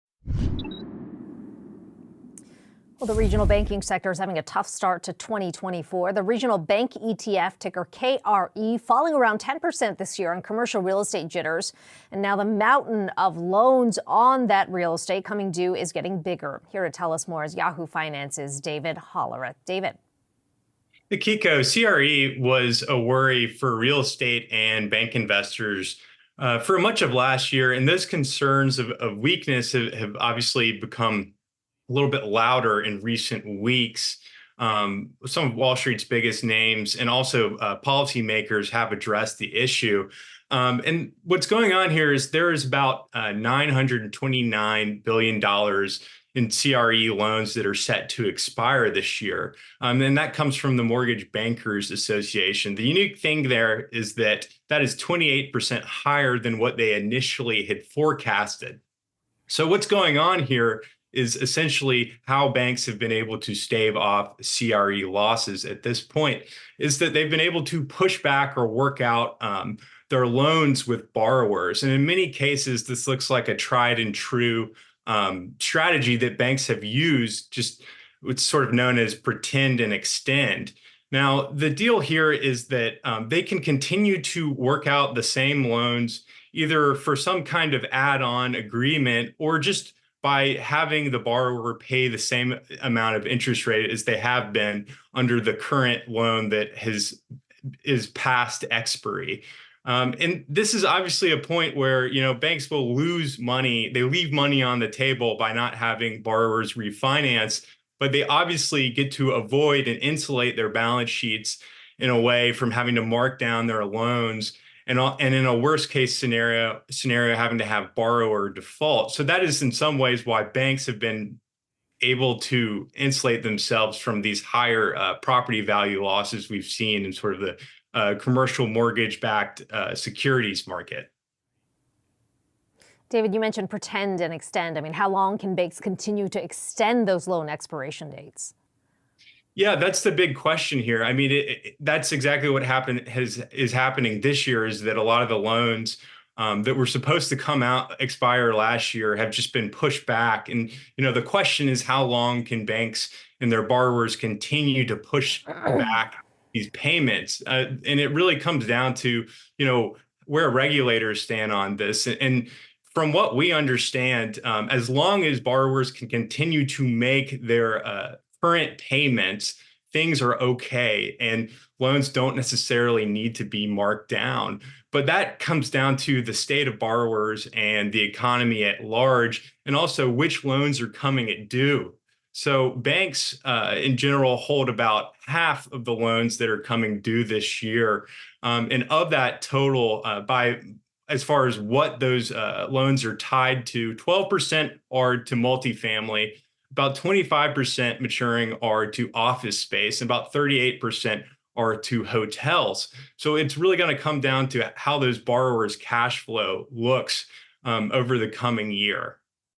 $929B worth of commercial real estate loans set to mature this year: Mortgage Bankers Association
About $929 billion worth of commercial real estate loans are set to mature this year, according to the Mortgage Bankers Association. It's an increase from the $659 billion the group previously forecasted.
Why did the estimate change so drastically?
Yahoo Finance's David Hollerith explains in the video above.